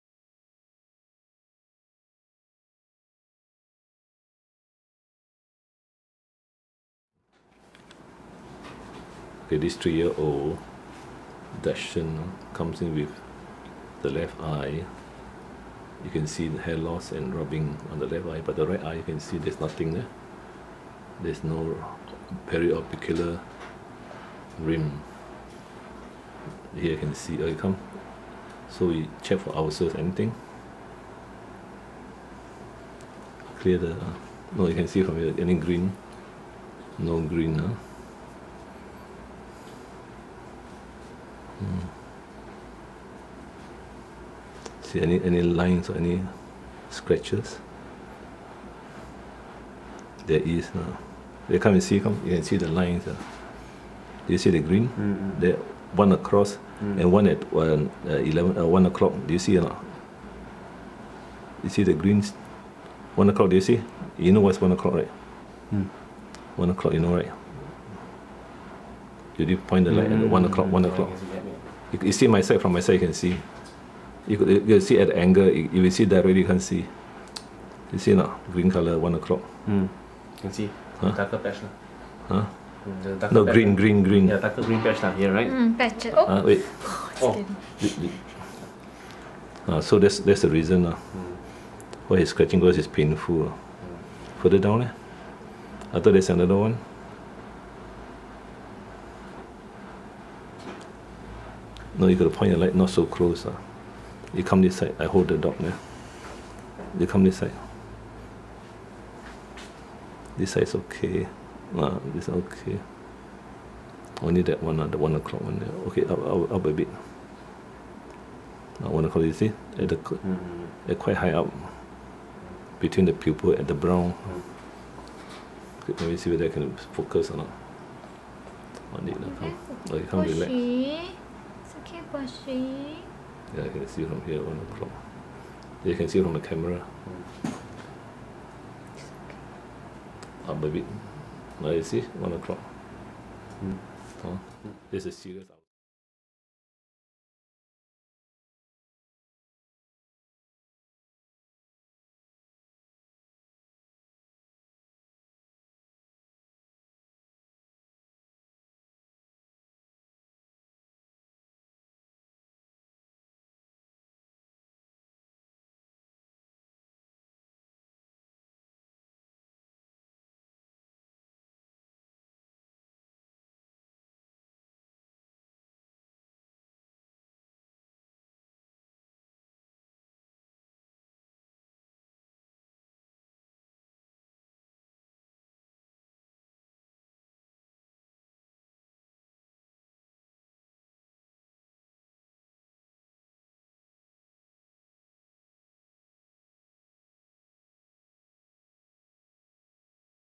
A 3-year-old Dachshund rubs his left eye frequently. Why?
Practise evidence-based veterinary medicine. Use
fluorescein drops or strip to show the owner that his
dog suffers from corneal ulcer as the ulcer will be stained green and visible.